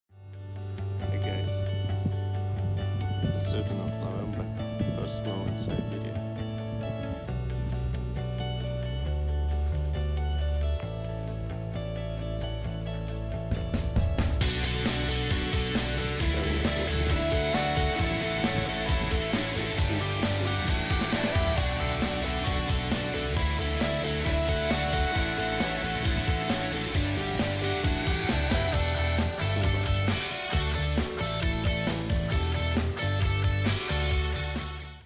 First snow, Christmas tree, cage, russian winter, Siberia
First snow in Siberia. Winter is coming, Chrismas tree. True Russia